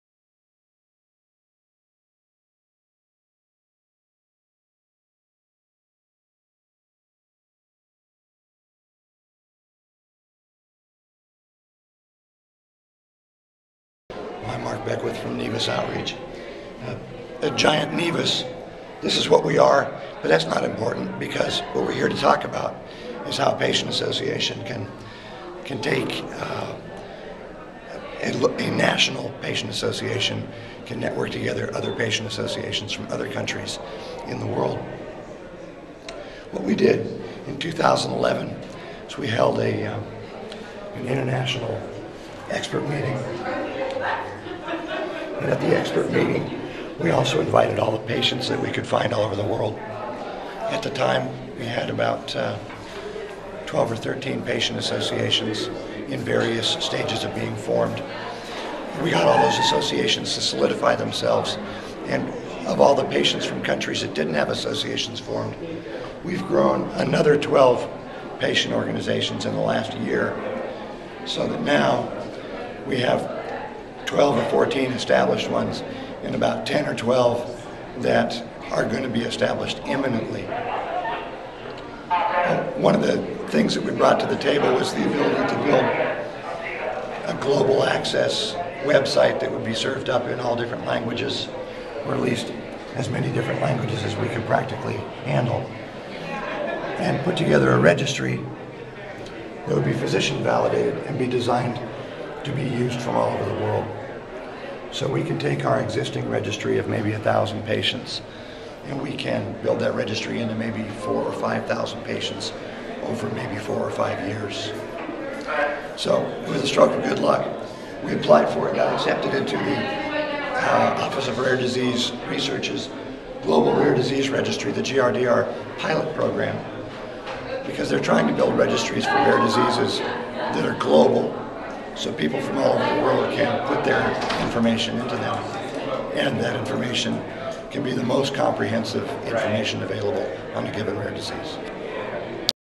mark beckwith vid
Mark Beckwith is the executive director of  Nevus Outreach Inc, an organization to Improve awareness and education about large congenital melanocytic nevi (CMN) and neurocutaneous melanocytosis (melanosis) (NCM). Nevus Outreach started out as an American patient advocacy group but quickly realized that it would be beneficial for patients with these rare conditions to combine scientific, clinical, advocacy, regulatory information with other CMN and NCM organizations from around the world. At the 2011 NEVUS annual meeting,
 33 representatives from 13 patient associations throughout the world were present to explore similarities and differences between their organizations and to begin the process of sharing information more openly.
Today, the Nevus Global Initiative  is up and running and their proposed physician-validated data and tissue repository has been accepted into the NIH's Global Rare Disease Registry (GRDR) pilot program, slated for completion of development by mid-2013.
The Nevus Global Initiative would not be possible without the generous assistance of the Morgan Family Foundation, the National Institute of Arthritis, Musculoskeletal and Skin Diseases (NIAMS-NIH), the Office of Rare Disease Research (ORDR-NIH), the University of Tübingen, and the members of all the congenital melanocytic nevus patient associations who donate time and effort to the Initiative.